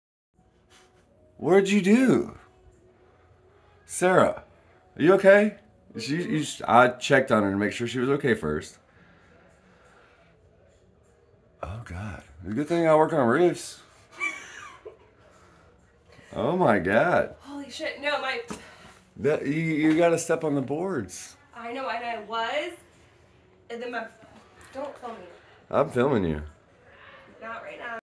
Woman fell through celling from attic
A woman sits on the floor in a pile of debris as there is a large hole in the ceiling above her which she fell through putting decorations back up in the attic of her house.